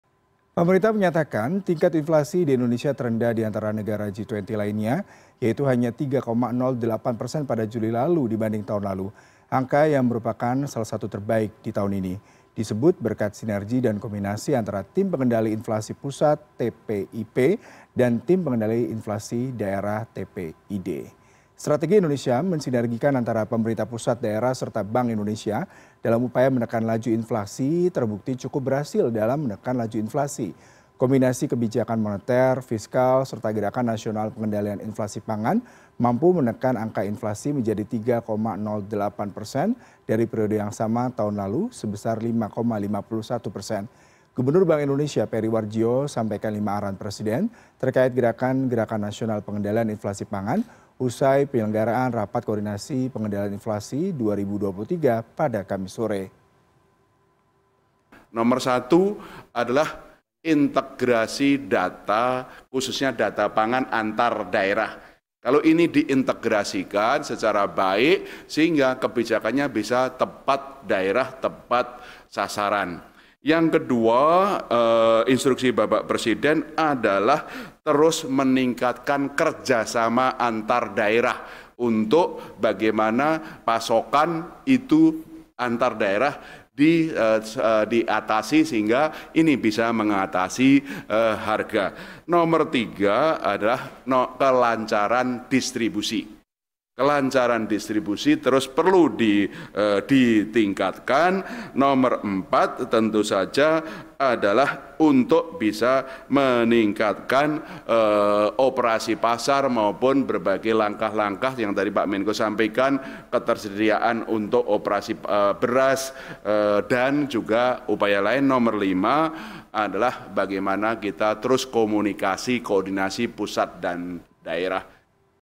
5 Arahan Jokowi untuk Kurangi Inflasi
Pemerintah menyatakan, tingkat inflasi di Indonesia terendah di antara negara G20 lainnya, yaitu hanya 3.08% pada Juli lalu dibanding tahun lalu. Angka yang yang merupakan salah satu terbaik di dunia ini, disebut berkat sinergi dan kombinasi antara tim pengendali inflasi pusat TPIP dan tim pengendali inflasi daerah TPID.